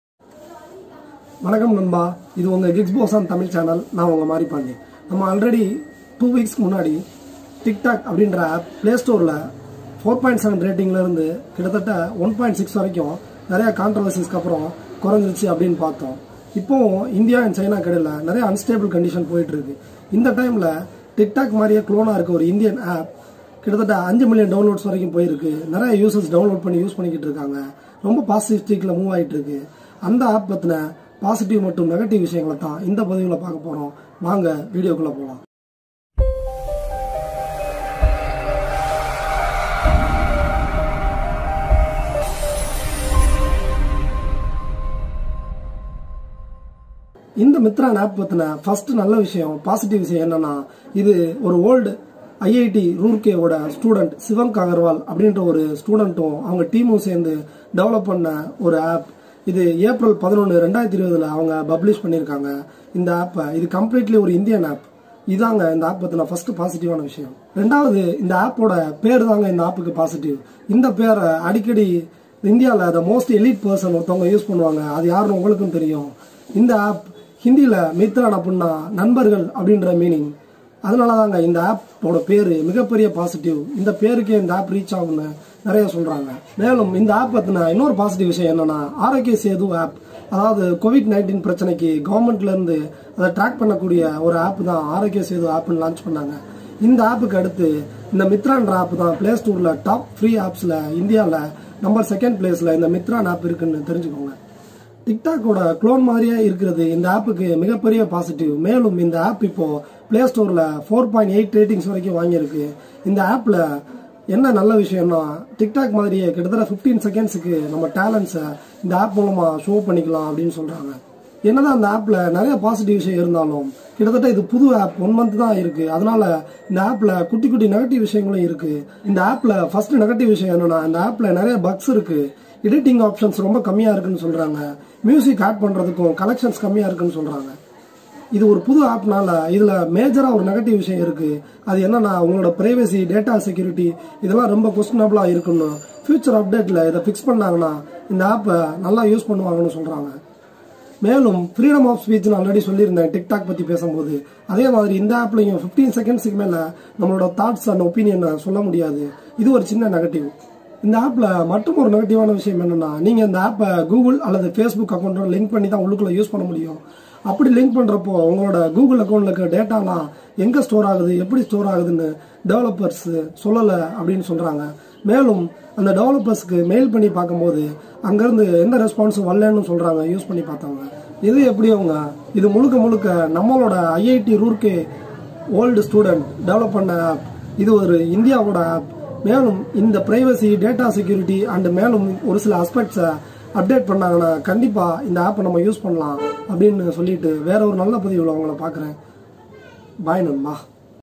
Mitron App Made in India🇮🇳 | Tik Tok App (Tamil)
All Features of Mitron app in Simple tamil Language